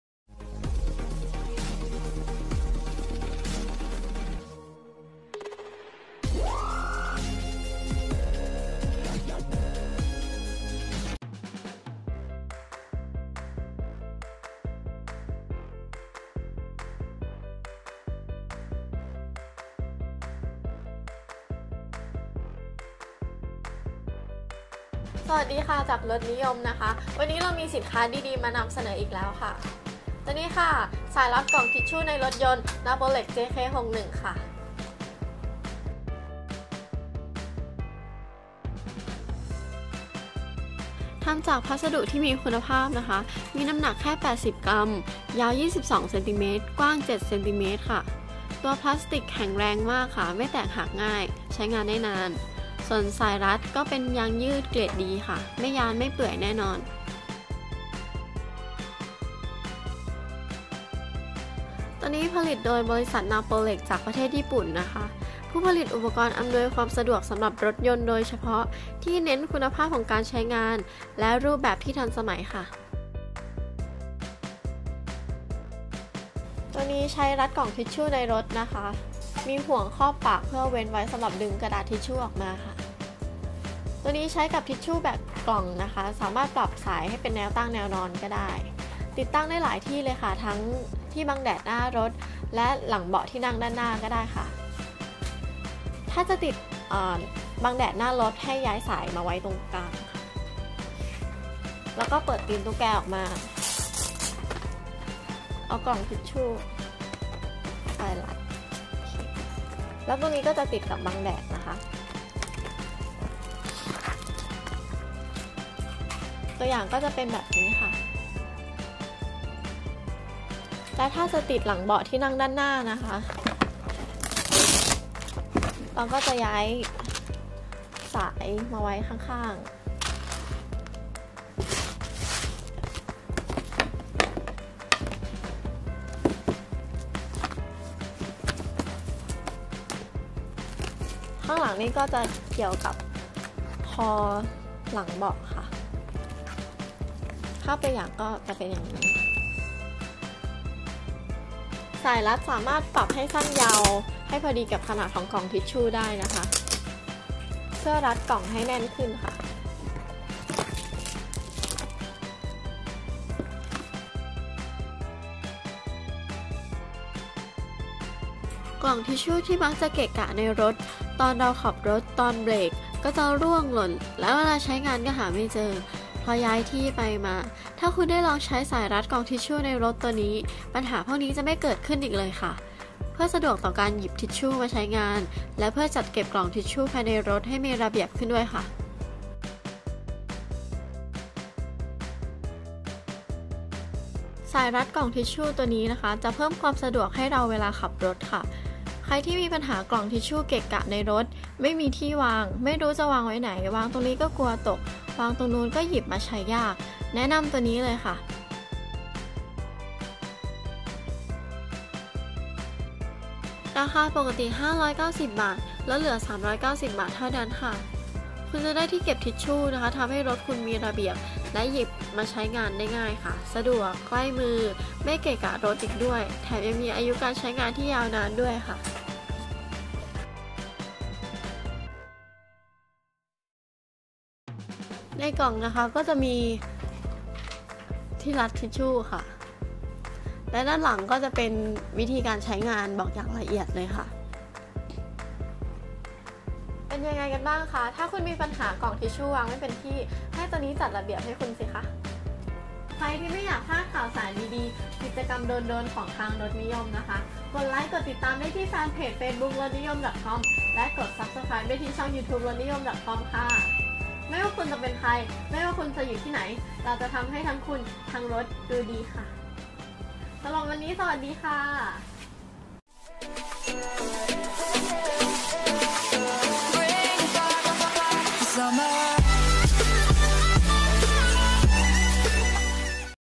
สายรัดกล่องทิชชู่ในรถ Napolex JK-61 | รีวิวสินค้า
Product description?
ทำจากพัสดุที่มีคุณภาพนะคะ มีน้ำหนักแค่ 80 กรัม ยาว 22 ซม. กว้าง 7 ซม. ตัวพลาสติกแข็งแรง ไม่แตกหักง่าย ใช้งานได้นานค่ะ ส่วนสายรัดก็เป็นยางยืดเกรดดี ไม้ยานไม่เปื่อยแน่นอน

Who produced it?
บริษัท napolex จากญี่ปุ่น ผู้ผลิตอุปกรณ์อำนวยความสะดวกสำหรับรถยนต์โดยเฉพาะ ที่เน้นคุณภาพของการใช้งาน และรูปแบบที่ทันสมัย

Product Feature?
ตัวนี้ใช้รักกล่องทิชชูในรถนะคะ มีห่วงครอบปากเพื่อเว้นไว้สำหรับดึงกระดาษที่ชูออกมา ตัวนี้ใช้กับทิชชูแบบกล่องนะคะ สามารถปรับสายเป็นแนวตั้งหรือแนวนอนได้ ติดตั้งได้หลายที่เลย ทั้งที่บังแดดหน้ารถ และหลังเบาะที่นั่งด้านหน้าก็ได้ค่ะ สายรัดสามารถปรับสั้นยาวให้พอดีกับขนาดของกล่องทิชชูได้นะคะ เพื่อรัดกล่องให้แน่นค่ะ

is it good for you?
กล่องทิชชูที่มักจะเกะกะในรถ ตอนขับรถตอนเบรกก็จะร่วงหล่น และเวลาจะใช้งานก็หาไม่เจอ เพราะย้ายที่ไปมา ถ้าคุณได้ลองใช้สายรัดกล่องทิชชูในรถตัวนี้ ปัญหาพวกนี้จะไม่เกิดขึ้นอีกเลยะคะ เพื่อสะดวกต่อการหยิบทิชชูมาใช้งาน และเพื่อจัดเก็บกล่องทิชชูให้ภายในรถมีระเบียบขึ้นด้วยค่ะ

who to use?
รถยนต์สำหรับหลายๆคนเป็นเหมือนบ้านหลังที่ 2 เลยนะคะ ดังนั้นจึงไม่แปลกที่จะมีของใช้อำนวยความสะดวก ที่จะเพิ่มความสะดวกให้เราเวลาขับรถนะคะ สายรัดกล่องทิชชูนี้ก็เหมือนกันค่ะ ใครที่มีปัญหากล่องทิชชูเกะกะรถ ไม่มีที่วาง ไม่รู้จะวางไว้ไหน วางตรงนี้ก็กลัวตก วางตรงนู้นก็หยิบมาใช้ยาก แนะนำตัวนี้เลยค่ะ

ราคาปรกติ 590 บาท ลดเหลือ 390 บาทเท่านั้นค่ะ คุณจะได้เก็บทิชชูที่ทำให้รถคุณมีระเบียบ และหยิบใช้งานได้ง่าย สะดวก ใกล้มือ และไม่เกะกะรถอีกด้วย แถมยังมีอายุการใช้งานที่ยาวนานด้วยค่ะ

What in thae box
ในกล่องนะค่ะ ก็จะมีที่รัดทิชชู่ ด้านหลังก็จะเป็นคู่มือบอกไว้อย่างระเอียดเลยค่ะ

อยากรู้จักกันก่อน
ทักมาเลยจ้าช่องทางนี้สะดวกสุดคร๊าบ…
▶ ใช้มือถืออยู่คลิก
หรือค้นหา ID "@rotniyom" ที่ LINE หรือ LINE@ (กรุณาใส่ "@" ด้วย) แล้วเพิ่มเป็นเพื่อนของคุณ — ที่ รถนิยม